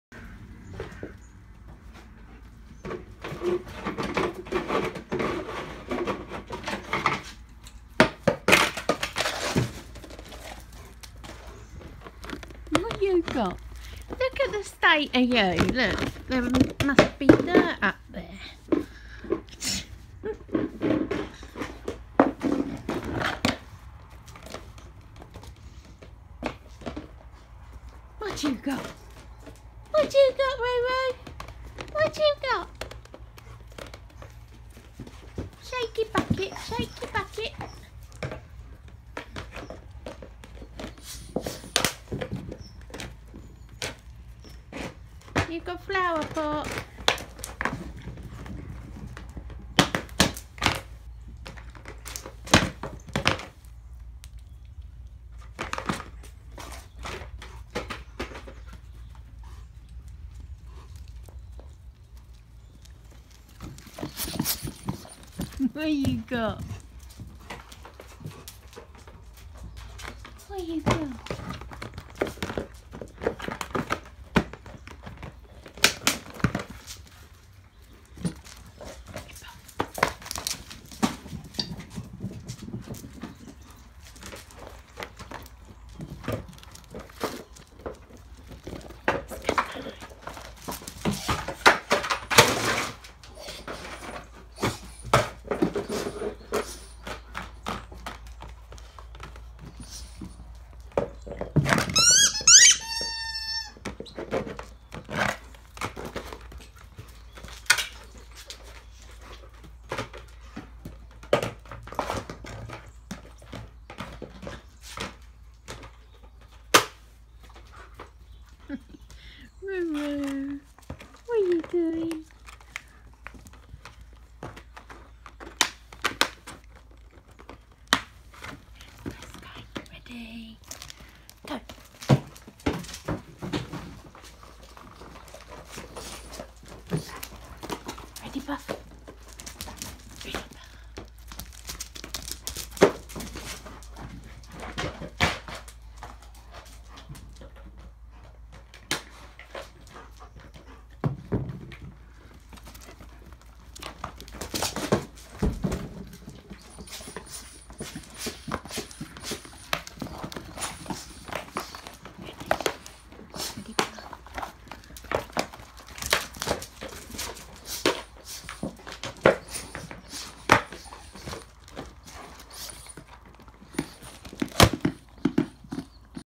German shepherd pack
Siberian husky German shepherd pup nicking flower pot 😁